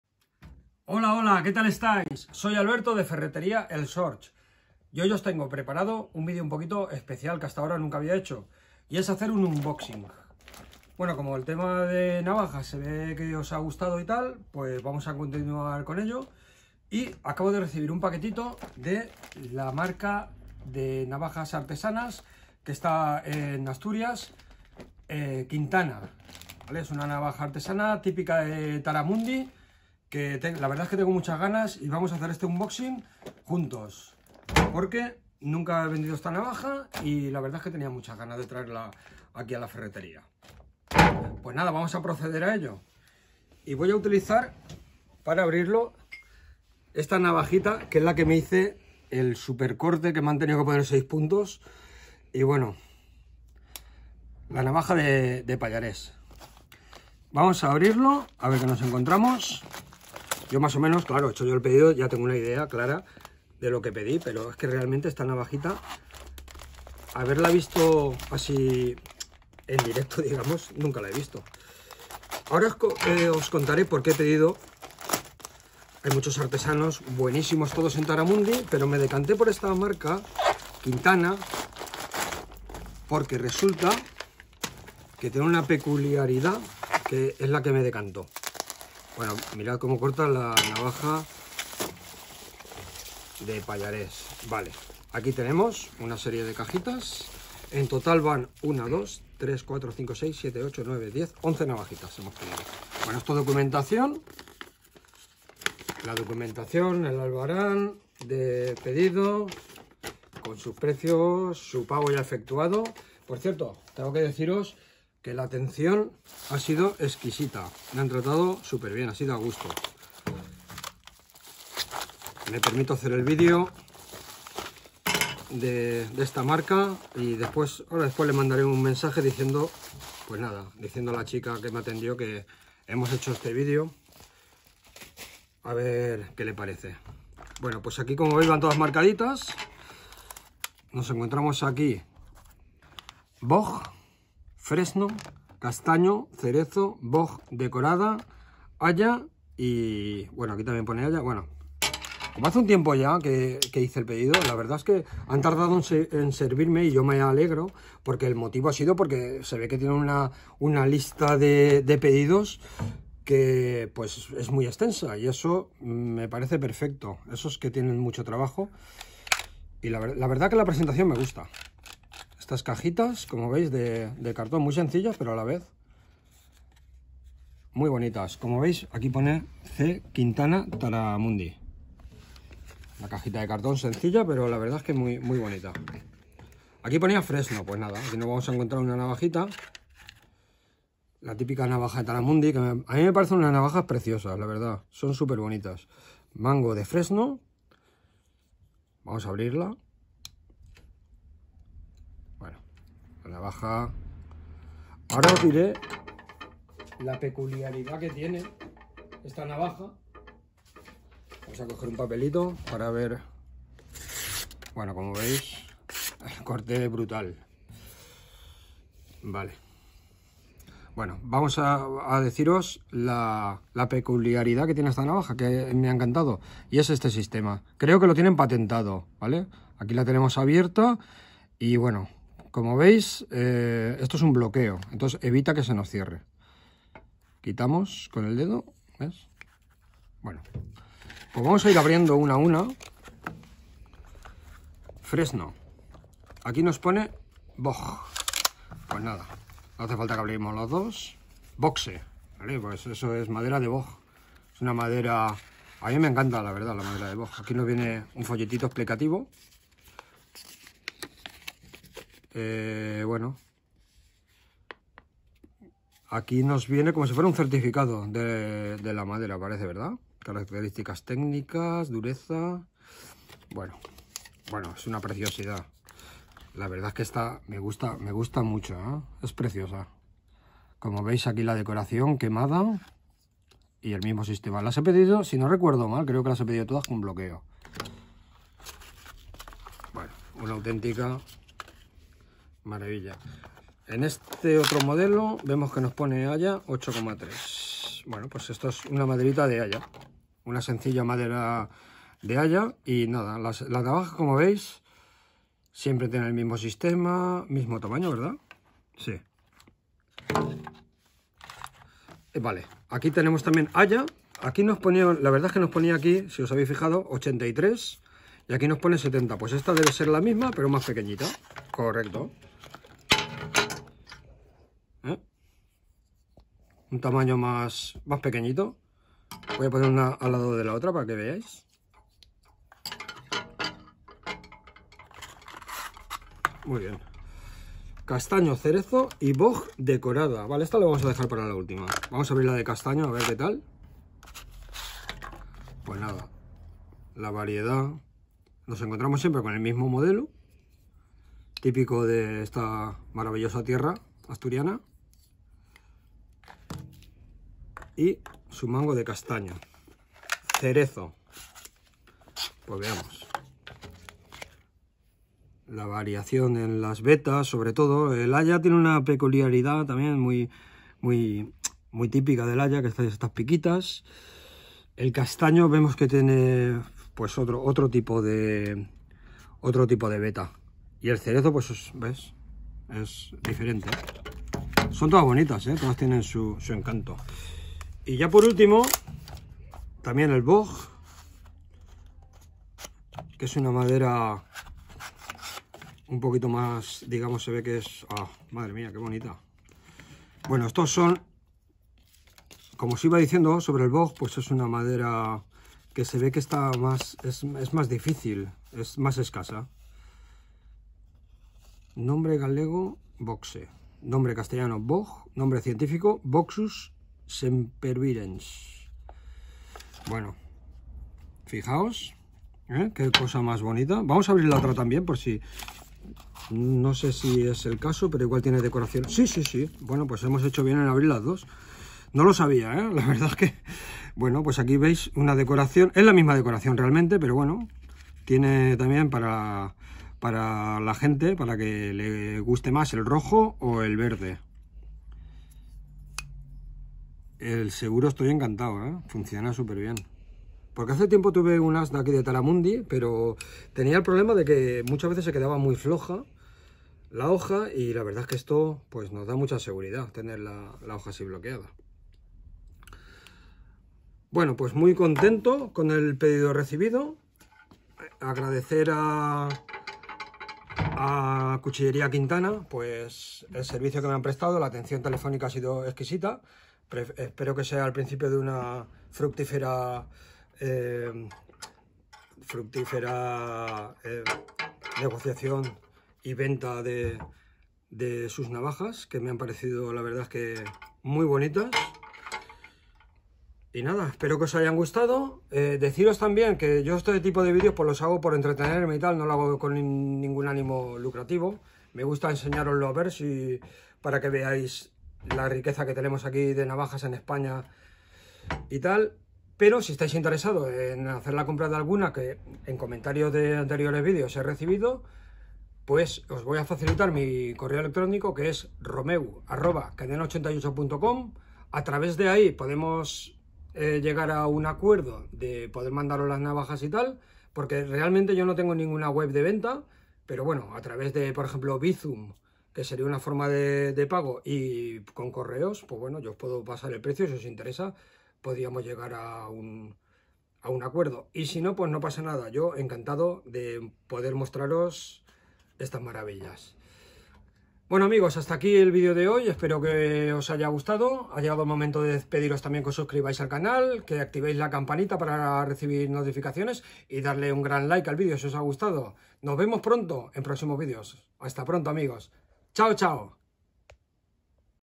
CARLOS QUINTANA_hablemos de navajas 3.0
Hola amig@s hoy abrimos juntos el paquete que me ha llegado de Asturias , perdonad por los fallos que puedan haber, pero se ha subido sin editar y a la primera salga como salga jajajaja , espero que os guste un saludo